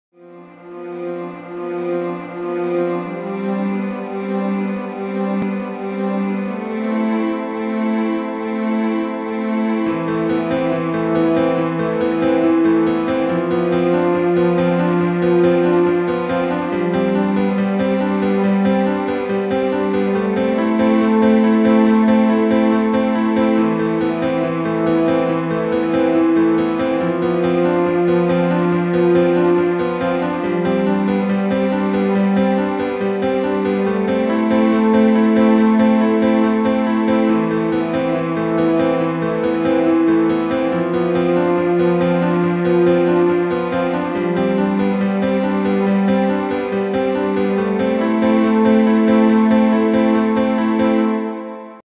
Point of View - Time Lapse Building Westend Plazza - Bratislava Construction
How has the view  between the years 3/2016 and 3/2018 changed.